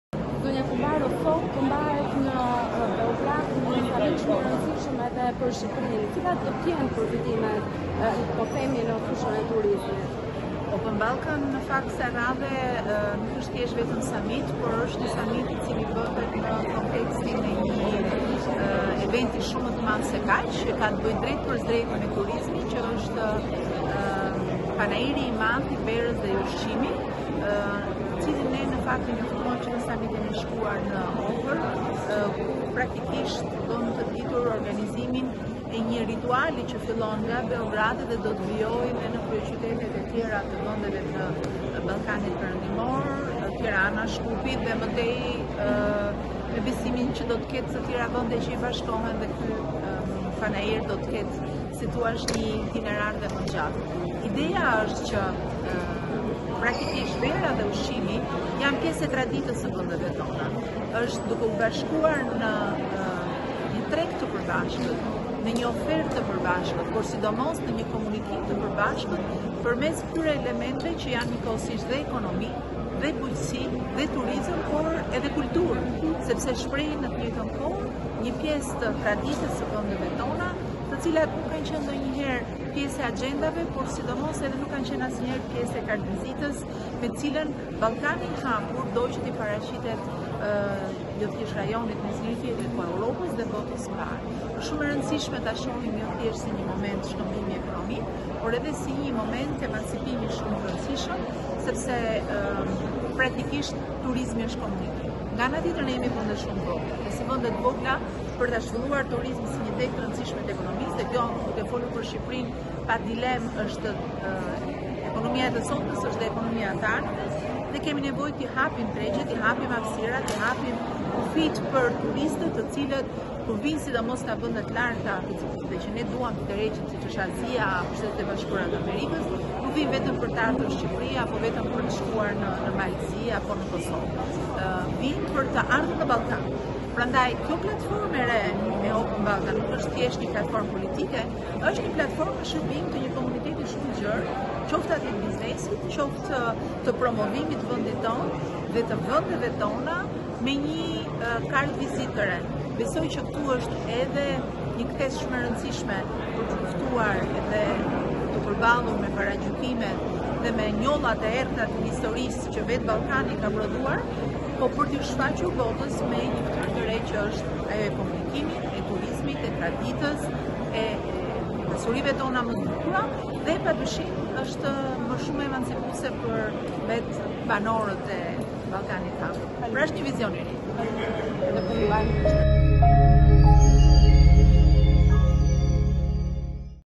Sa i rëndësishëm është Ballkani i hapur për turizmin? Ministrja për Report Tv: Moment emancipimi
reportTV lajmeshqip lajme Ministrja e Turizmit Mirela Kumbaro e pyetur nga gazetarja e Report Tv, Danjela Maloku ka theksuar se Ballkani i Hapur nuk duhet parë thjesht si shkëmbim por si një emancipim shumë të rëndësishëm pasi turizmi në vetvete është ekonomi.

‘Një samit është në kontestin që bëhet pikërisht për turizmin. Që në samitin e shkuar ne praktikisht bëmë të ditur një ritual që filloi nga Beogradi dhe do të vijojë me vende të tjera të Ballkanit Perëndimor. Ideja është se praktikisht vera dhe ushqimi janë tradita jonë dhe këto elementë po vendosen në ekonomi, kulturë etj. Shprehin në vetvete gjëra që nuk kanë qenë ndonjëherë pjesë e axhendës, me të cilën Ballkani i hapur do që ti paraqitet jo thjesht vendeve të rajonit por edhe vendeve të tjera të Evropës. Pra, nuk është thjesht një shkëmbim ekonomik, si moment emancipimi shumë të rëndësishëm sepse praktikisht turizmi është ekonomi. Vendi tona janë të vogla dhe kanë nevojë të hapin turizmin e tregjet.’- tha Kumbaro.